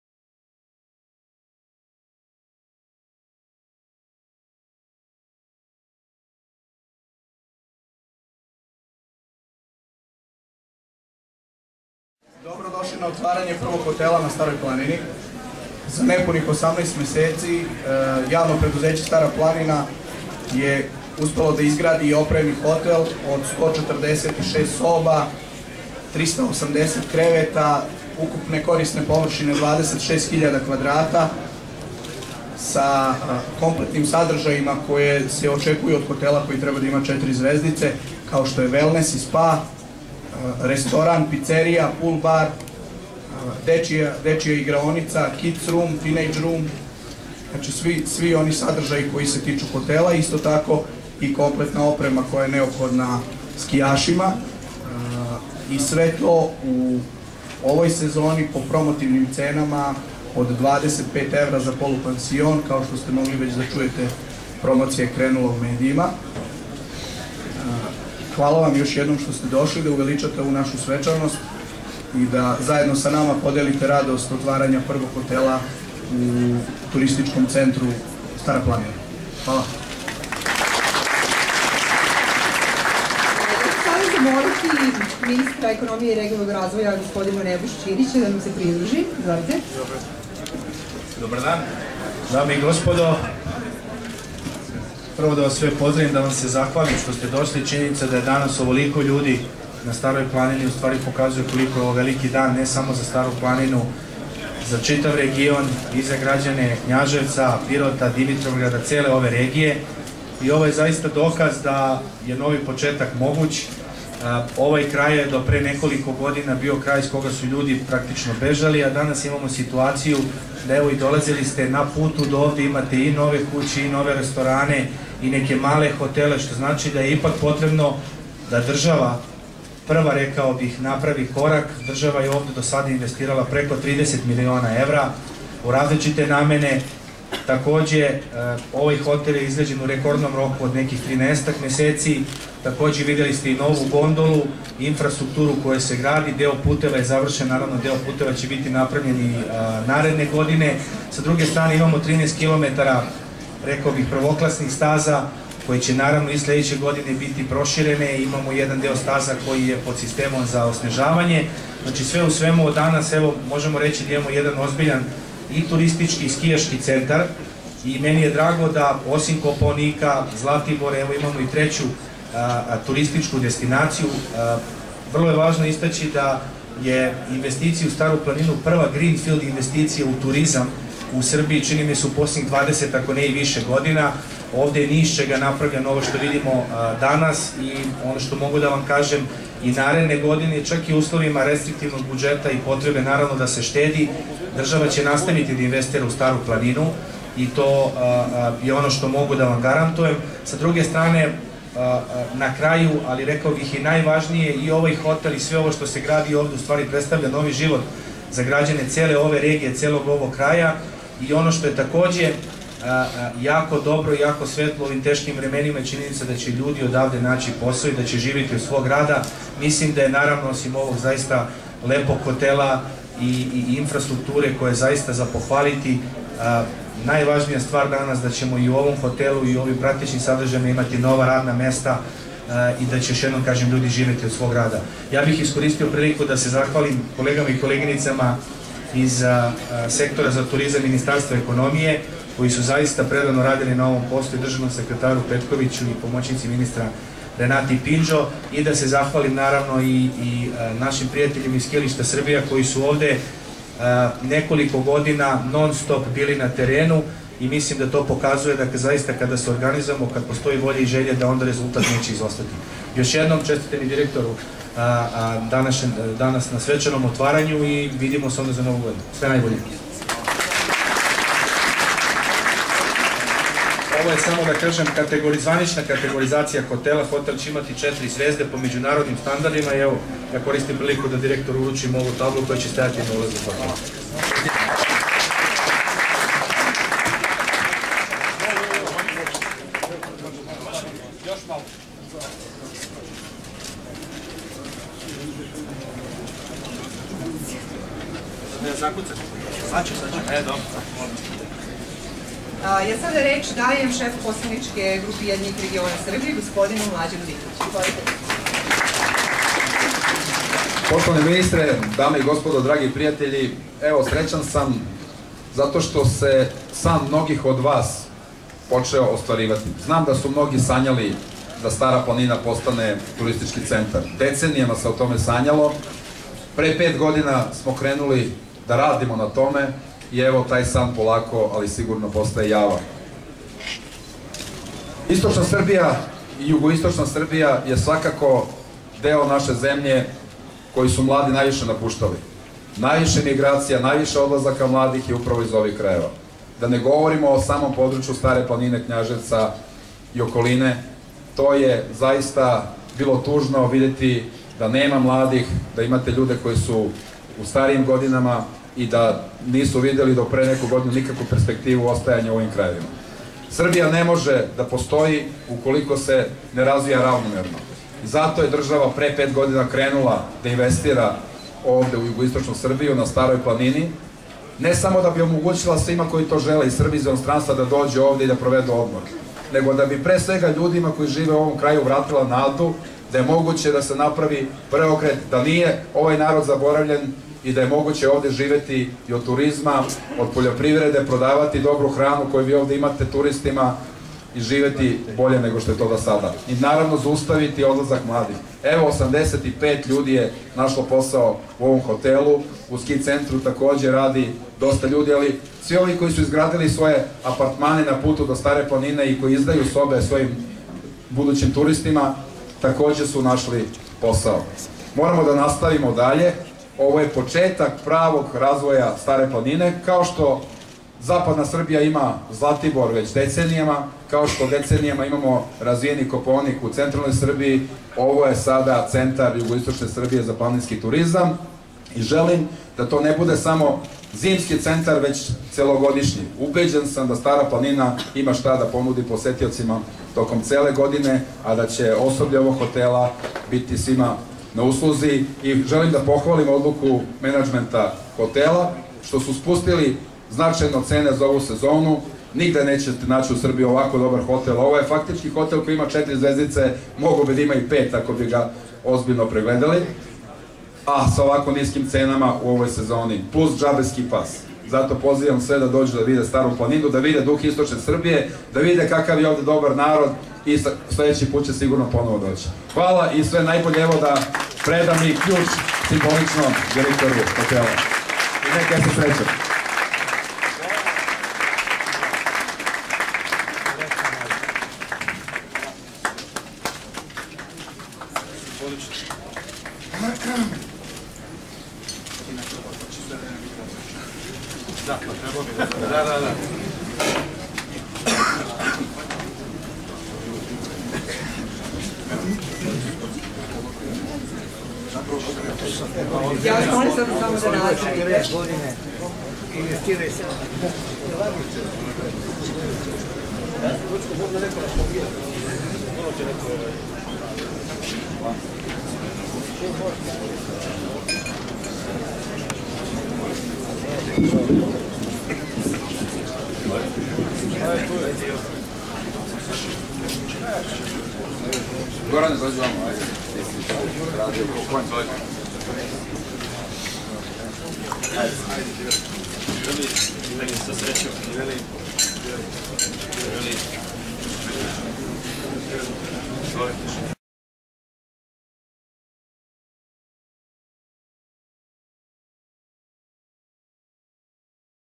Snimak otvaranja hotela na Staroj planini
Snimak otvaranja prvog hotela na Staroj planini -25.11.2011 godine, uz prisustvo brojnih zvanica - iz naše bogate  Arhive 💯🎞️🔝🎥